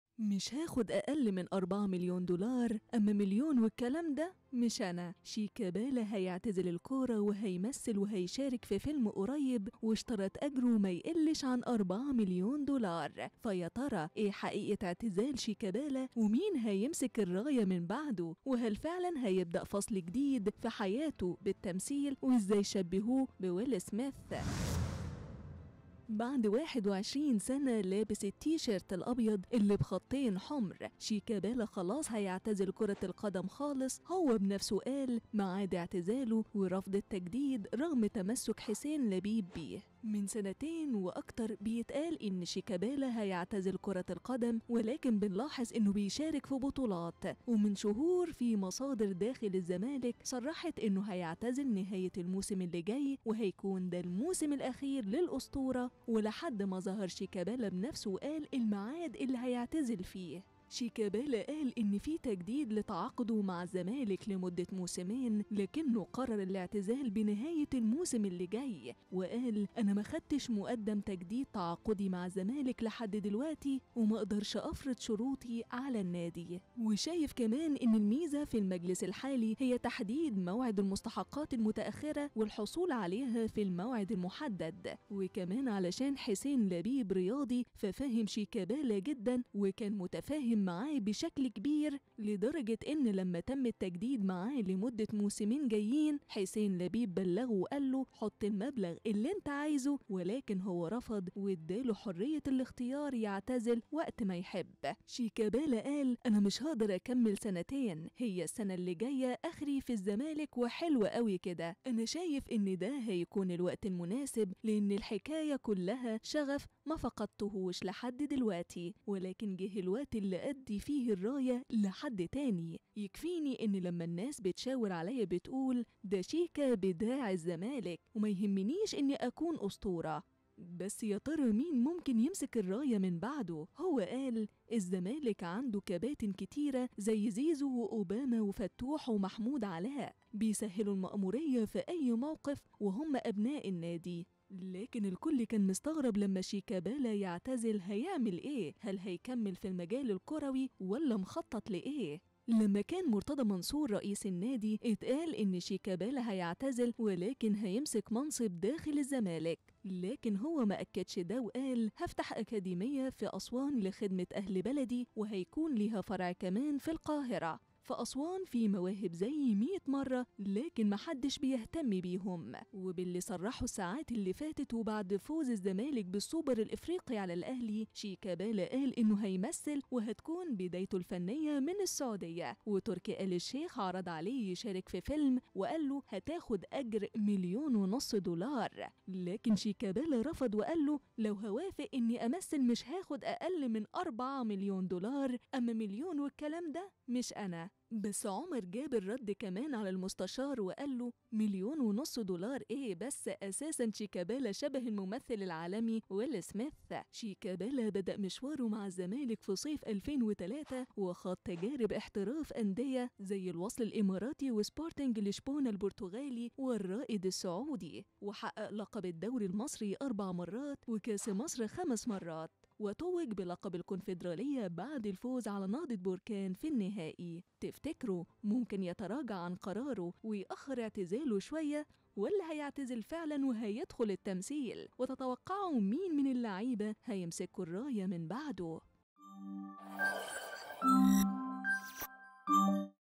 هياخد 4 مليون دولار!.. شيكابالا هيدخل التمثيل بعد اعتزاله: ليه مش هيكمل فى المجال الكروى؟
من سنيتن وأكتر بيتقال إن شيكابالا هيعتزل كرة القدم ولكن بنلاحظ إنه بيشارك فى بطولات ،ومن شهور فى مصادر داخل الزمالك صرحت إنه هيعتزل نهاية الموسم الجاى  وهيكون ده الموسم الأخير للأسطورة ولحد ما ظهر شيكابالا بنفسه وقال المعاد اللى هيعتزل فيه ..